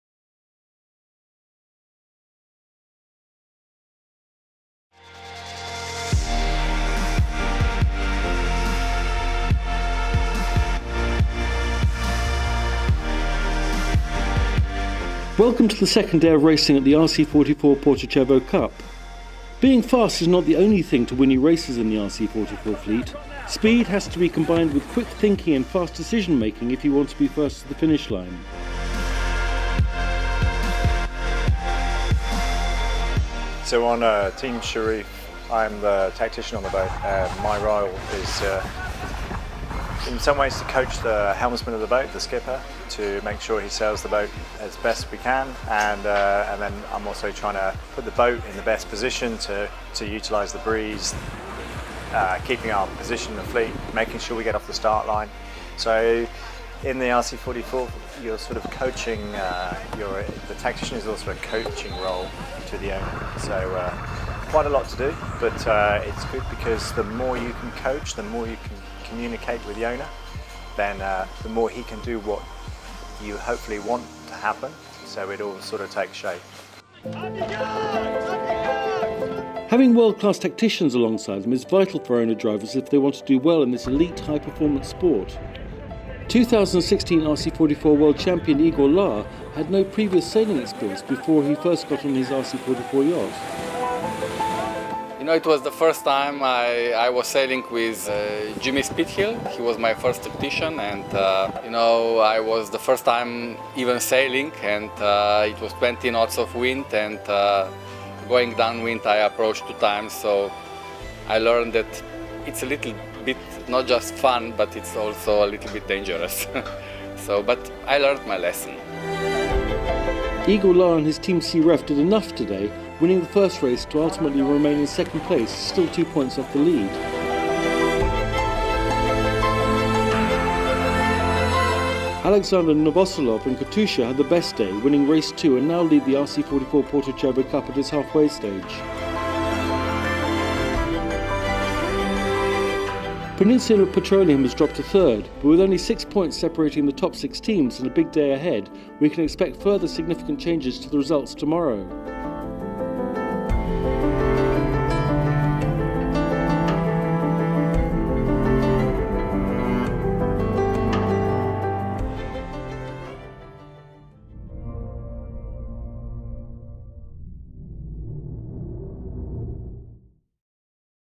Day 2 RC44 Porto Cervo Cup - Katusha ahead after blustery high scoring day
Uncertainty continued about the weather for day two of the RC44 Porto Cervo Cup, hosted by the Yacht Club Costa Smeralda. In the event, competitors used most of their sails as, after a light to moderate start, the wind kicked in close to the class’ maximum limit and there were several significant shifts. Thanks to all this, there was plenty of excitement and many many lead changes, but it also proved a high scoring day for the nine RC44s.

The RC44 Porto Cervo Cup is the second event of the 2017 RC44 Championship Tour, which pits the world's top businessmen and the best sailors in the industry compete in high-performance one-design 44-foot carbon racing yachts.